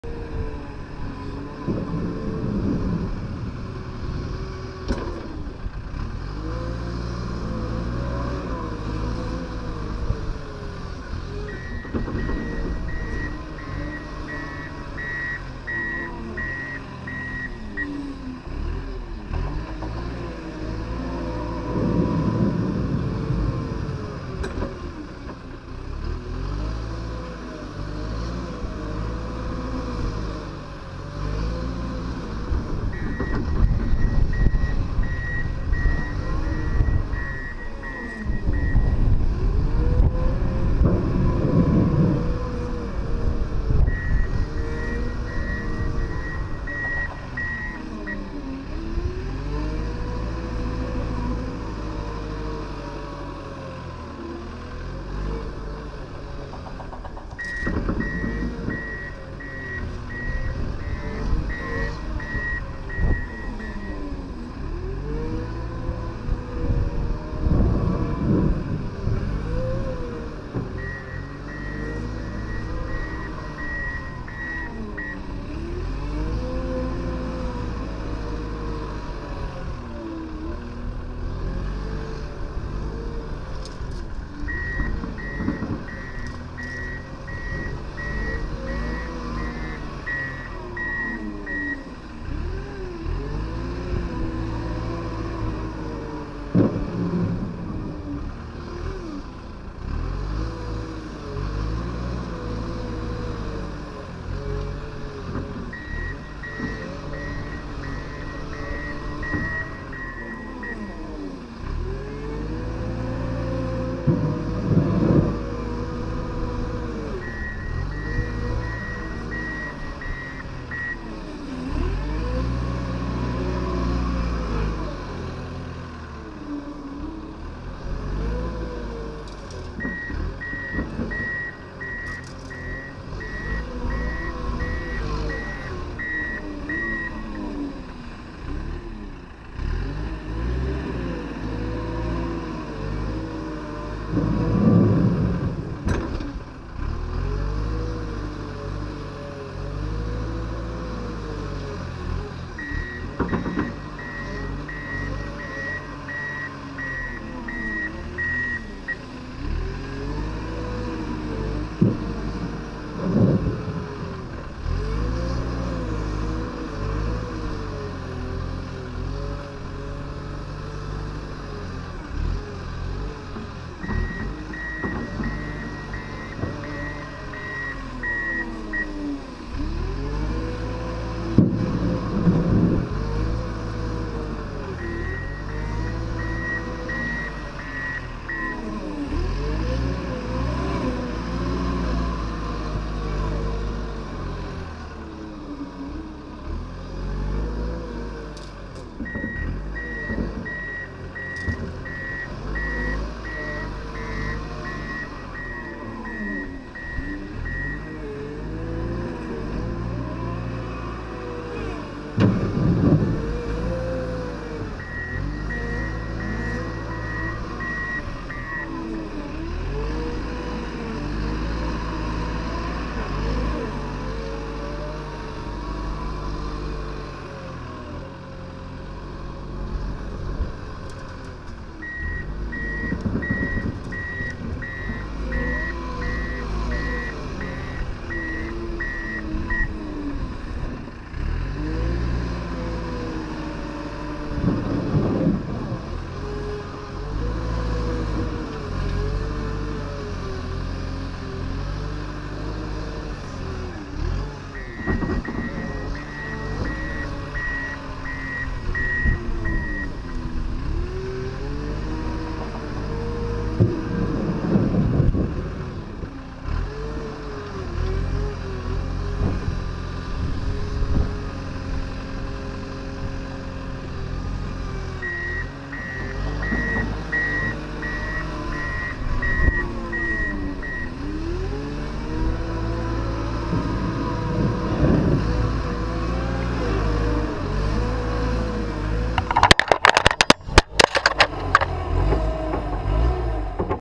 Komatsu WA200 Pushing Back Snow Banks
Pushing back snow banks the day after a storm at a large commercial property. The snow was taking up parking spots so it had to be moved.

Like what you see? Check out The Dirt Ninja on Facebook